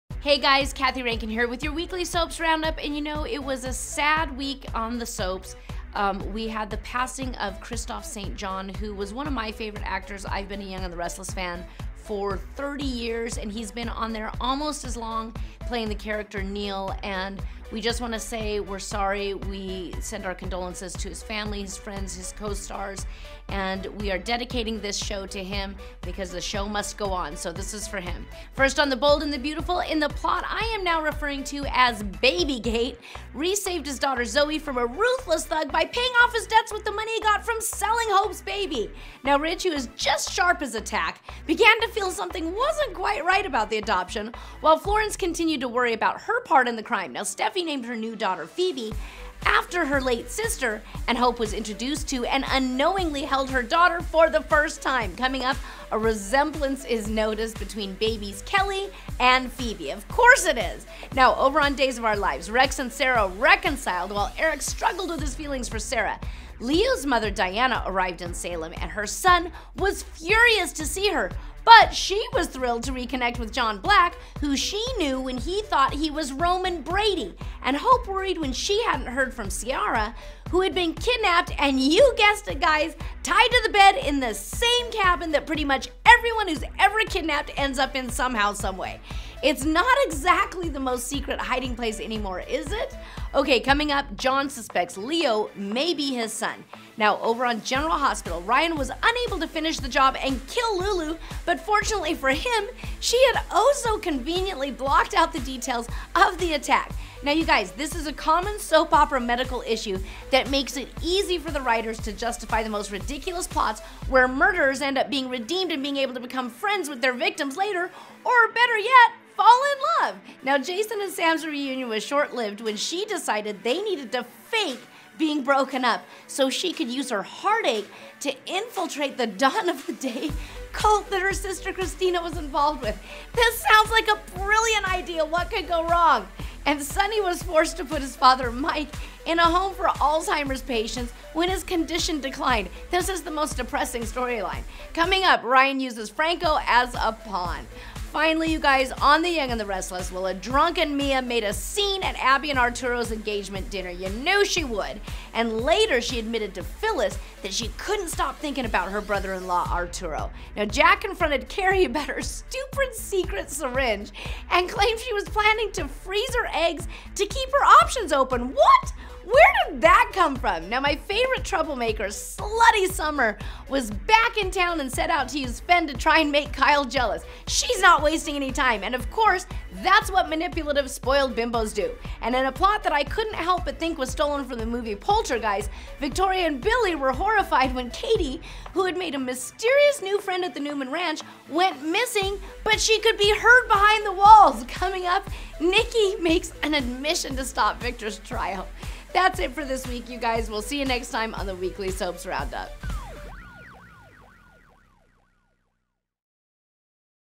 Soaps.com Weekly RoundUp - Week of January 28th - February st, 2019
Here's the latest Soaps.com recap on everything you missed on last week's Soap operas!
Get the latest Soaps on Soaps.com

First on “The Bold and the Beautiful,” Reese saved his daughter Zoe from a ruthless thug by paying off his debts with the money he got from selling Hope’s baby. Ridge began to feel something wasn’t right about the adoption, while Florence continued to worry about her part in the crime. Steffy named her new daughter Phoebe, after her late sister, and Hope was introduced to and unknowingly held her daughter for the first time. Coming up: A resemblance is noticed between babies Kelly and Phoebe.

Over on “Days of our Lives,” Rex and Sarah reconciled, while Eric struggled with his feelings for Sarah. Leo’s mother Diana arrived in Salem, and her son was furious to see her, but she was thrilled to reconnect with John Black, who she knew when he thought he was Roman Brady. And Hope worried when she hadn’t heard from Ciara, who had been kidnapped and tied to the bed in the same cabin everyone who’s kidnapped ends up in. Coming up: John suspects Leo may be his son.

And on “General Hospital,” Ryan was unable to finish the job and kill Lulu, but fortunately for him, she had blocked out the details of the attack, a common soap opera medical issue. Jason and Sam’s reunion was short-lived when she decided they needed to fake being broken up so she could use her heartache to infiltrate the Dawn of Day cult that her sister Kristina was involved with. And Sonny was forced to put his father Mike in a home for Alzheimer’s patients when his condition declined. Coming up: Ryan uses Franco as a pawn.

Finally, on “The Young and the Restless,” a drunken Mia made a scene at Abby and Arturo’s engagement dinner, and later admitted to Phyllis that she couldn’t stop thinking about her brother-in-law Arturo. Jack confronted Kerry about her secret syringe, and she claimed she was planning to freeze her eggs to keep her options open. Summer was back and set out to use Fen to try and make Kyle jealous. And Victoria and Billy were horrified when Katie, who had made a mysterious new friend at the Newman ranch, went missing and could be heard behind the walls! Coming up: Nikki makes an admission to stop Victor’s trial.